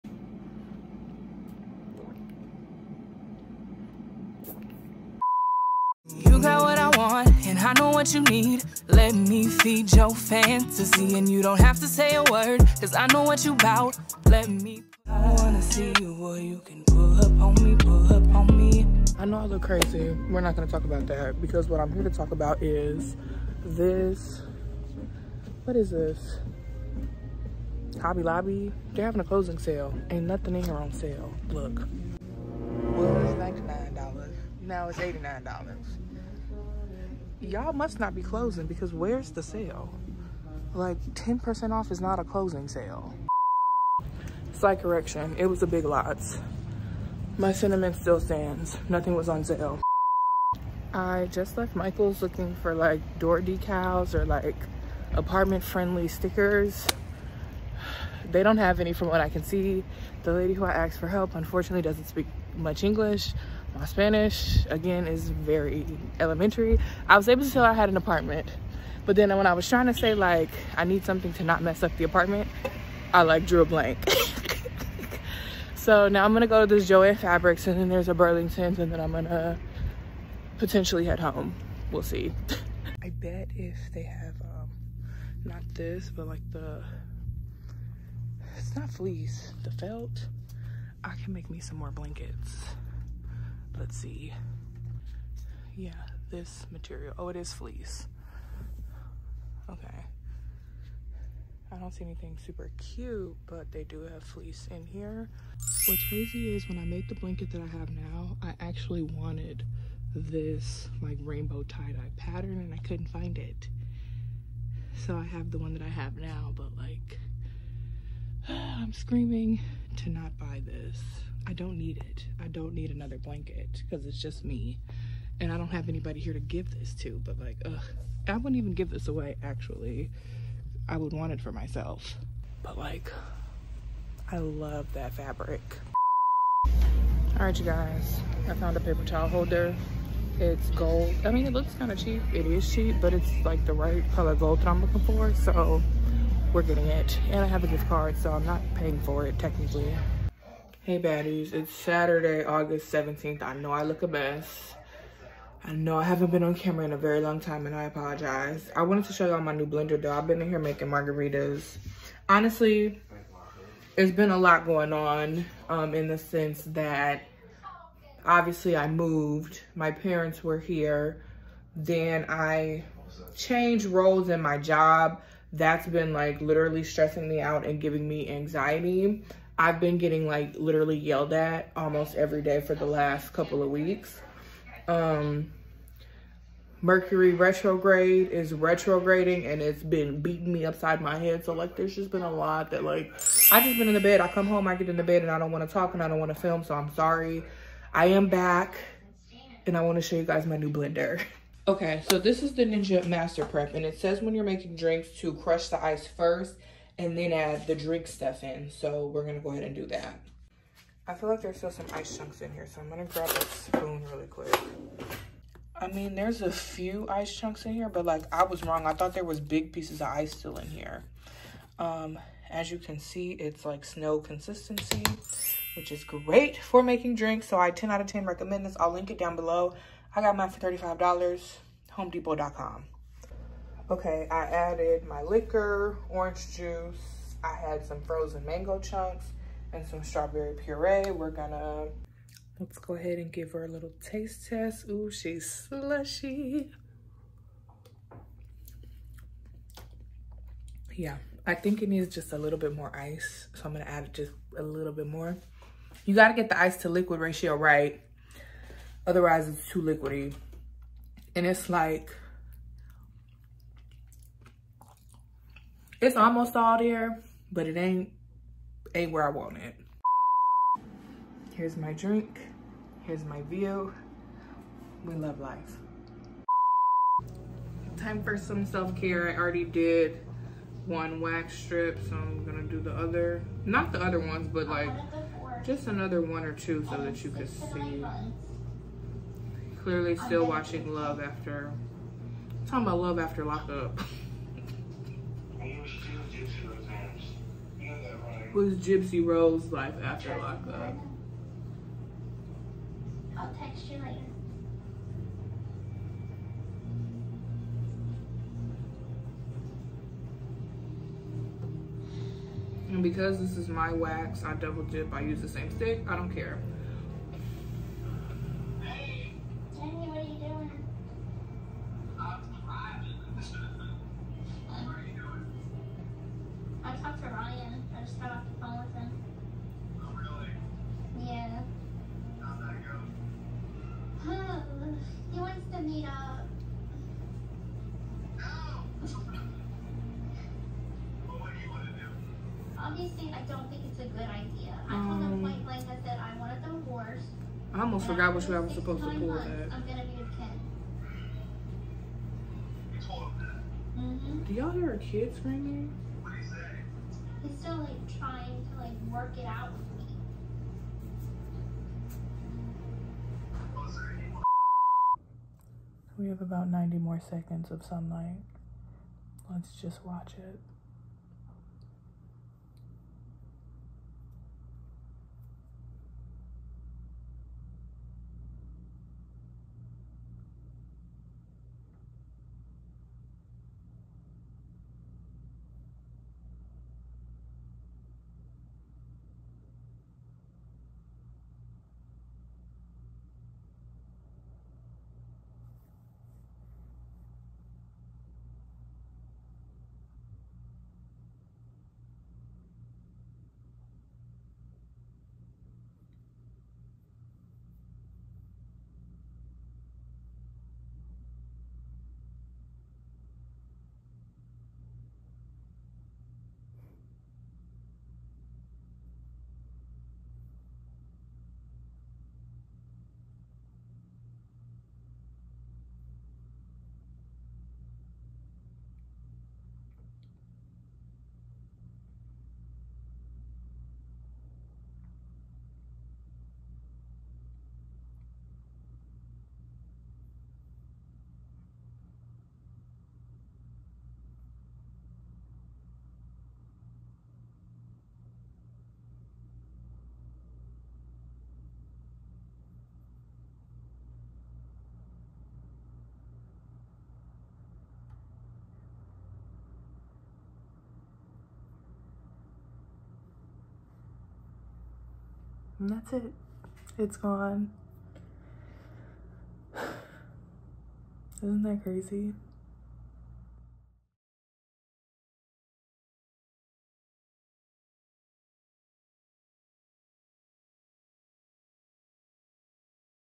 SATURDAY Vlog: Life in Miami (Just to Keep My Posting Streak) Week 20
Life has sucked as of lately. I took a huge break from vlogging however the content must go on. So here's a video literally just to keep my streak going.

AMAZON STOREFRONT

Ninja Master Prep
*Home Depot